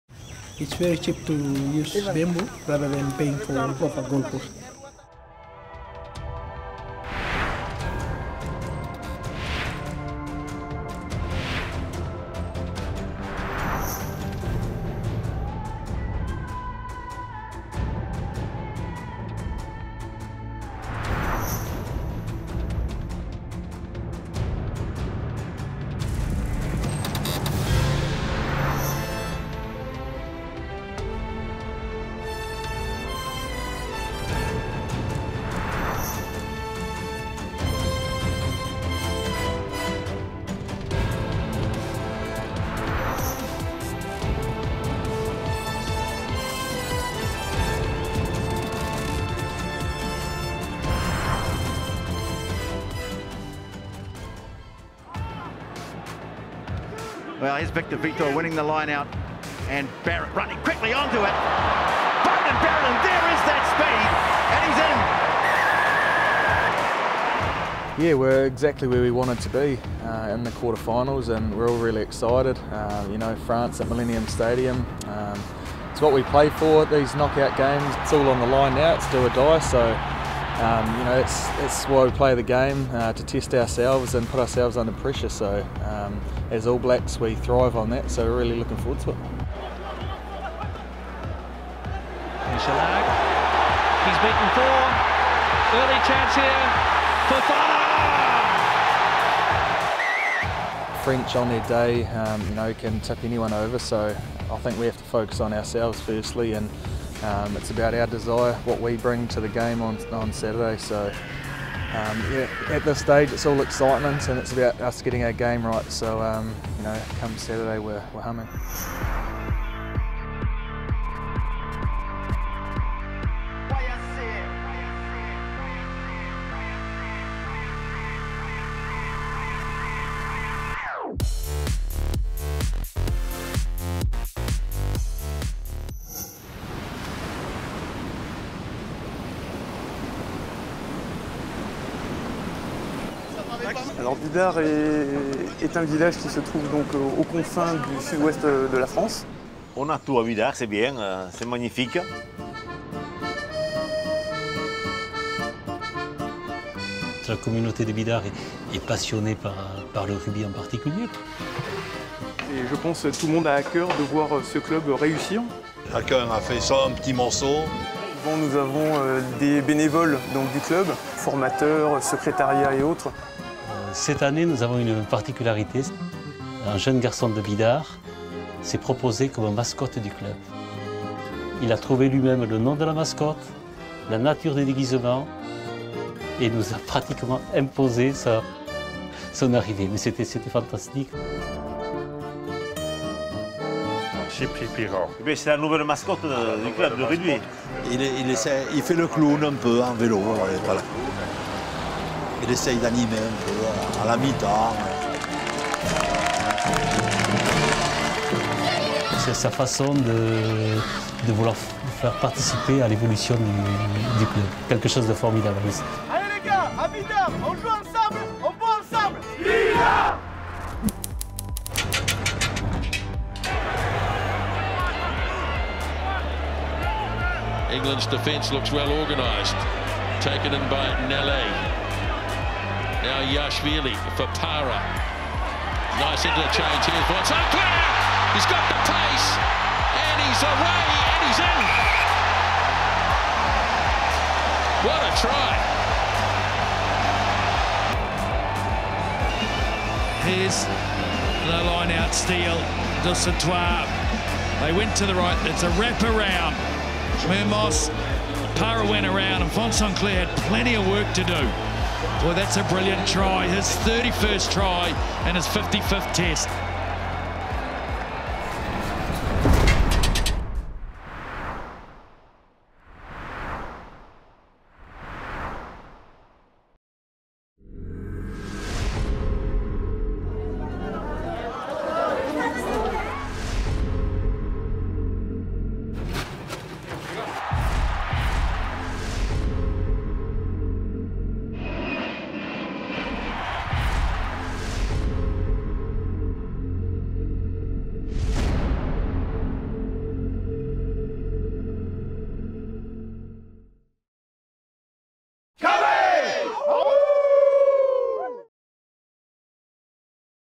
Vincent Clerc's legendary try - RWC Daily
Episode 34 of our daily video round-up of the Rugby World Cup 2015 in association with Land Rover. As a weekend of RWC Quarter Finals draws ever nearer, we look back at a legendary QF try as Vincent Clerc scores a try against England in 2011. France face a difficult test against New Zealand this weekend.